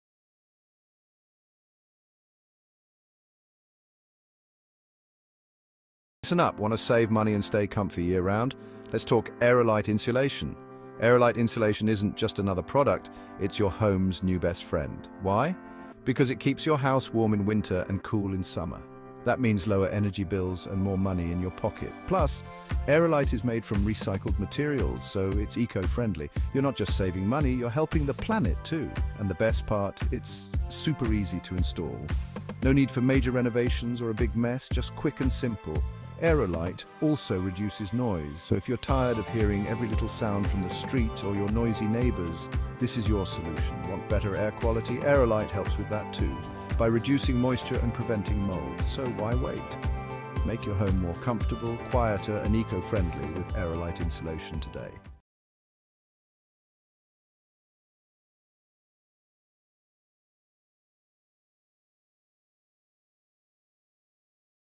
Why Aerolite Insulation is A Game Changer For Your Home
This is gonna blow your mind... … Imagine your home being cooler in summer and warmer in winter! Meet Aerolite Insulation – your home’s new best friend. …

With Aerolite, indoor temperatures drop by 5 to 8 degrees during those scorching summer months. … And guess what? It keeps your home 5 to 8 degrees warmer in winter too! …

But that’s not all! Aerolite isn’t just about temperature control. It’s got stellar noise reduction with an NRC of 1.1, making your home quieter and more serene. …

Plus, it’s non-combustible, SANS compliant, and SABS approved, so you know it’s safe and reliable. … This new Soft Touch Aerolite is not just effective, but comfortable to handle. And, say goodbye to pests, because it doesn’t support vermin! …

With its long lifespan, you’re in for lifelong energy savings. … So why settle for ordinary when you can have extraordinary comfort? …

Upgrade to Aerolite Insulation today and enjoy a cooler summer, a warmer winter, and a quieter home – all while saving on energy costs. …

Your home deserves the best, and Aerolite delivers! …